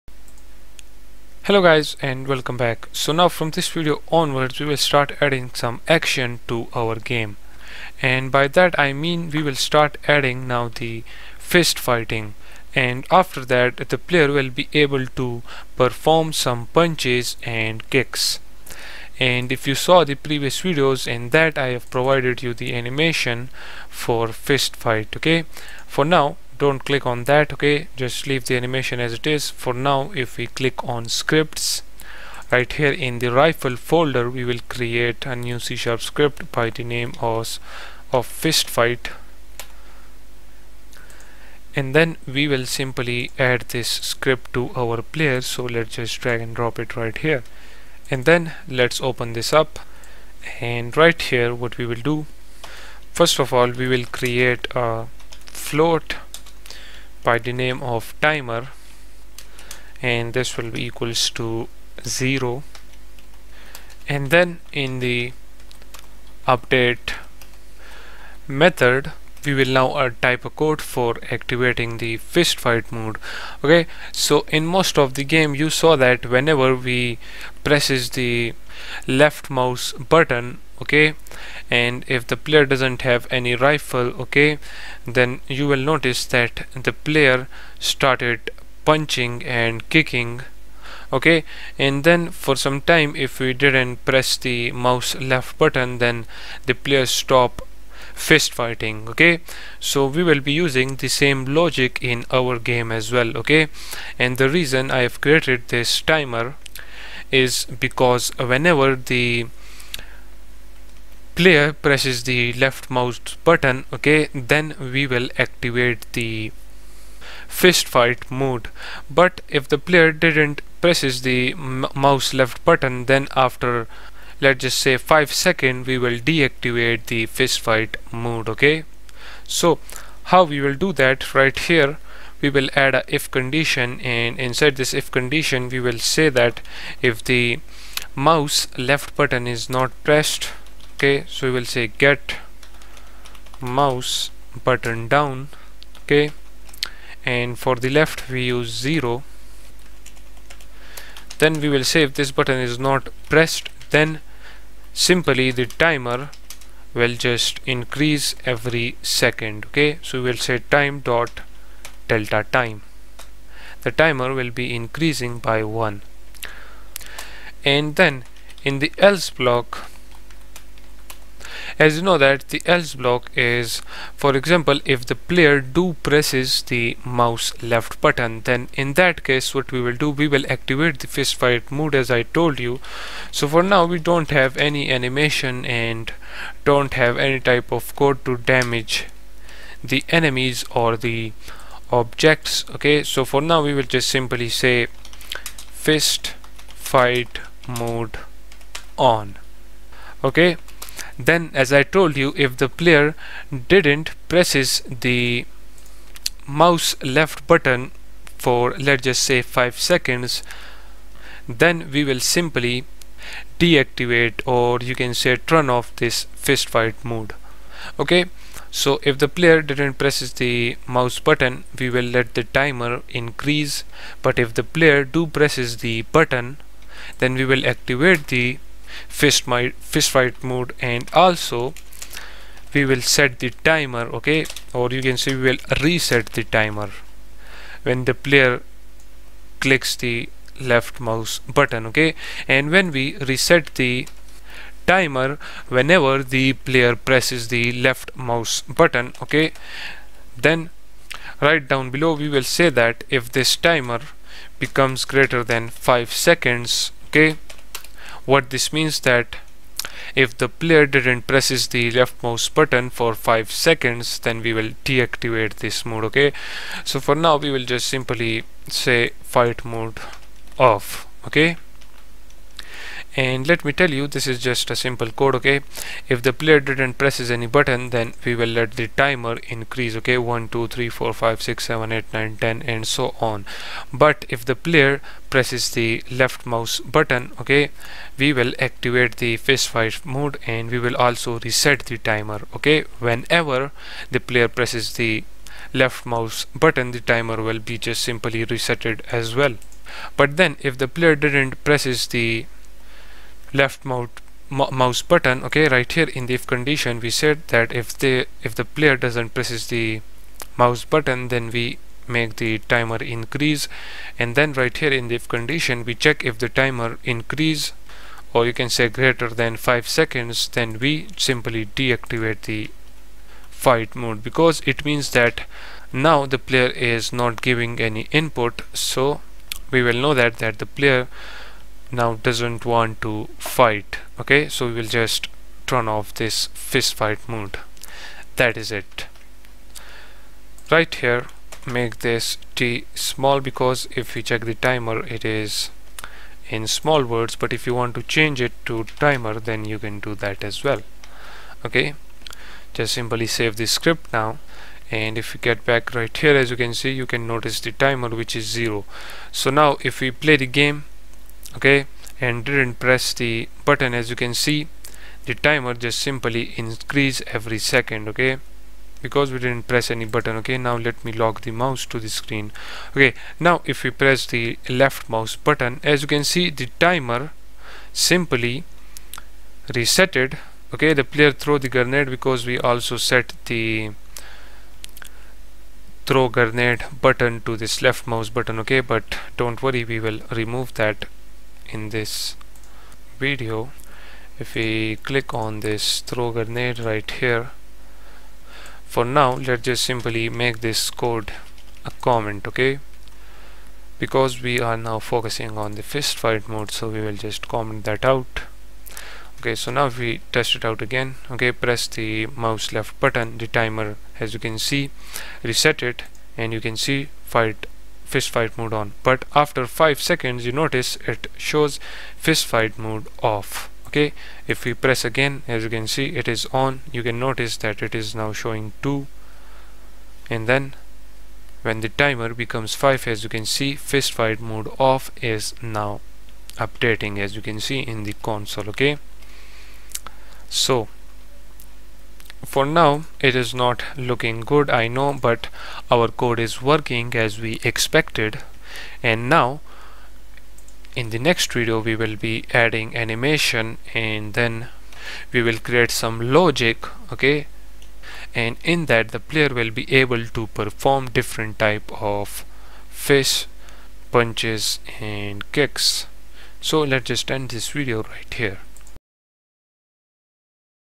Fist Fight Unity 3d Game Tutorial | Realistic Building Games for Android & iOS Course 2023
This is video tutorial# 45 of unity 3d assassin's creed mirage game clone course. In this course you will learn and develop assassin's creed mirage game clone using unity game engine.